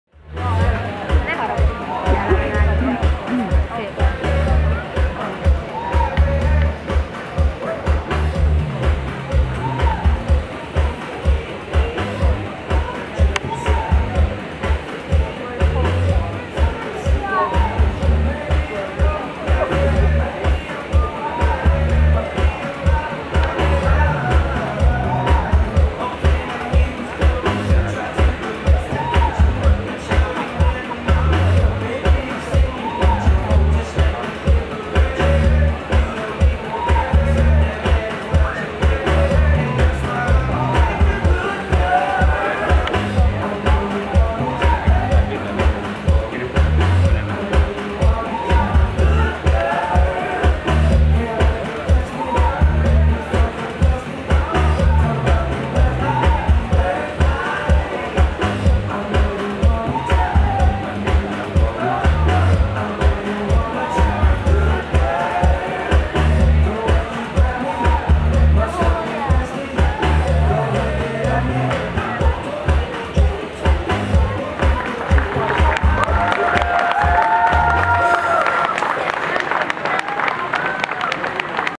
C&M Dance moderntánc 1 - Balatoni Szezonnyitó 2014 Siófok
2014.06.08.-án, Pünkösdkor, a Balatoni Szezonnyitón, a C&M Dance TSE moderntánc bemutató műsora.
(Érdemes a „minőséget" (felbontást) teljes képernyős módban, jobboldalt, alul 1080p-re (v. 720p-re) állítani, a megfelelő technikai minőségi hatás végett.)